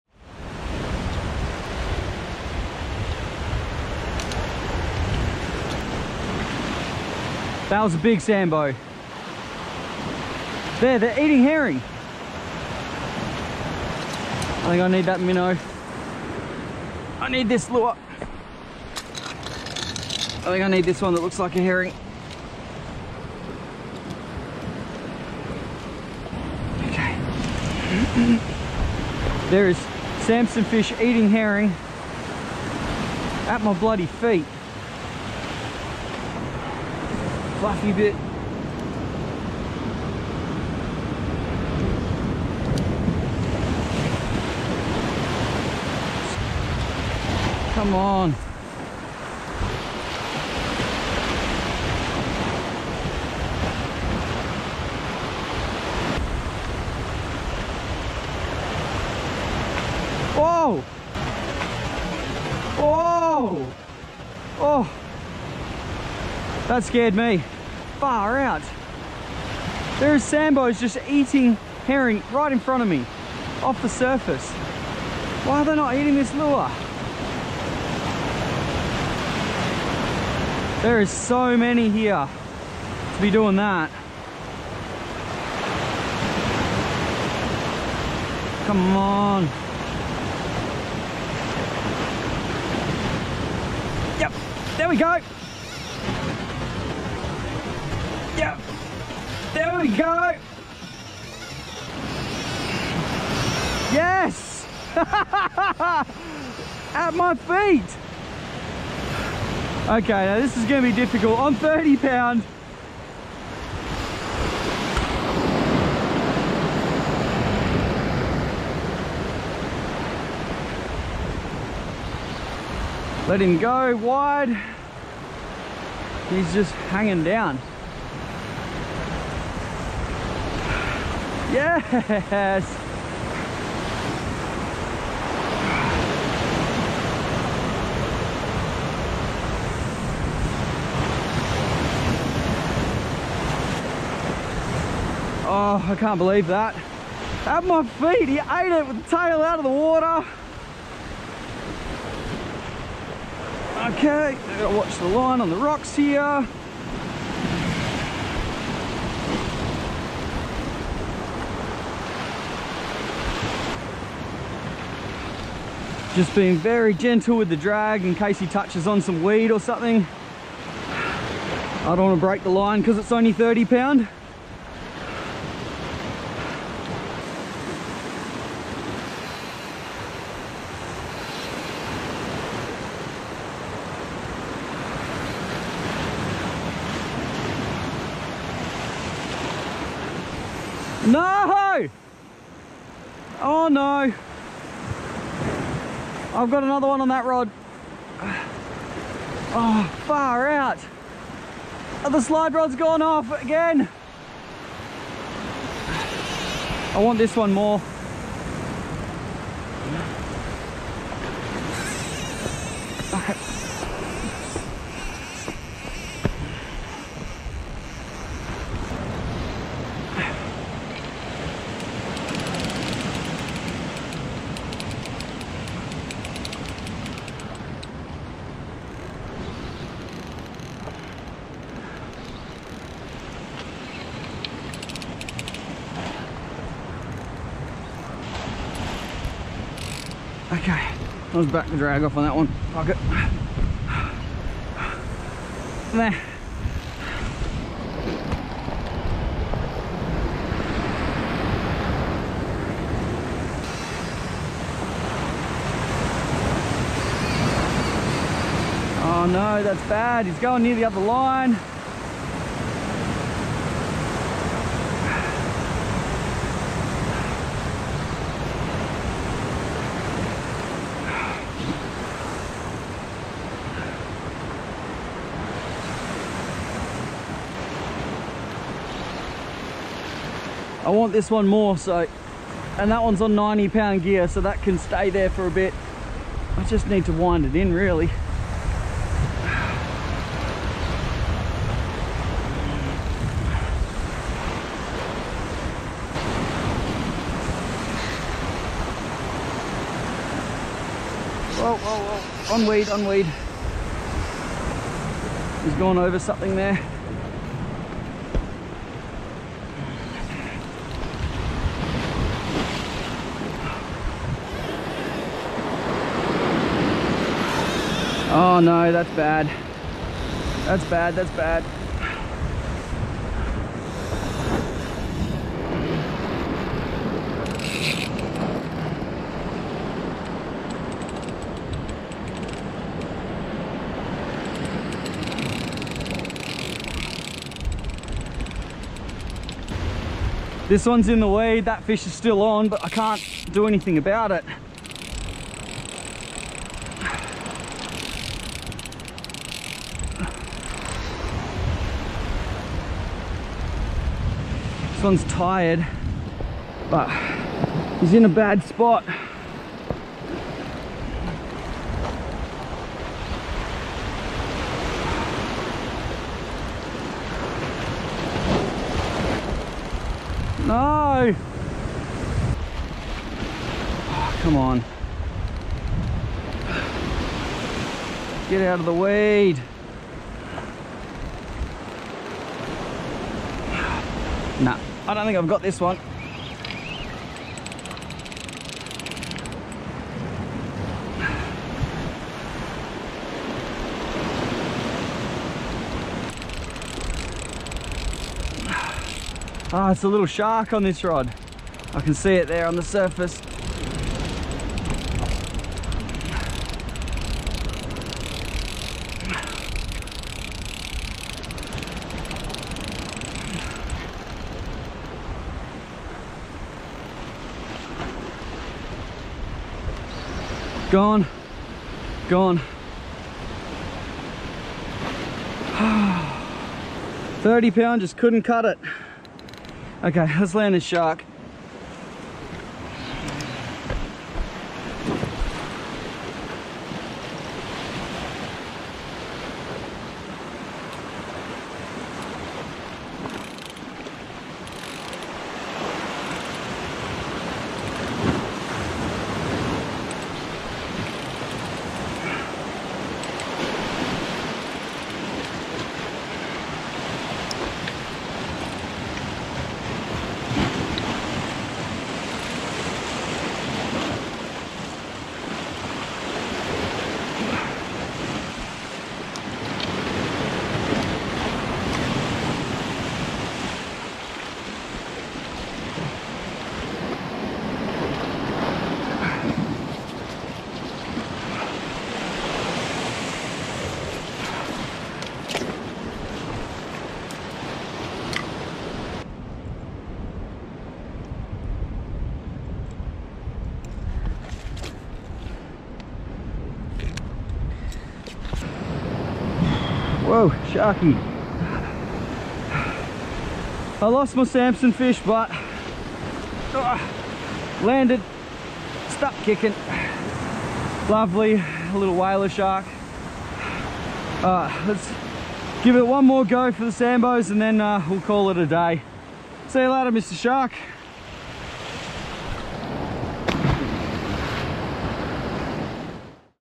It Ate It At My Feet
Crazy session chasing Samson fish on my own and managed to hook one then have my other rod go off and have to fight both fish at once.

Gear Used:

PE8 Setup,

 @DaiwaAustralia  Saltiga 14000XH, Sensor Wave 1303H, PE8 Saltiga 12 Braid.

30lb Spin,

Certate SW 6000XH, Seajigger 315M, 30lb J Braid Grand.

Hooks from BKK, 7/0 Beastly Cat and 4/0 Viper Trebles.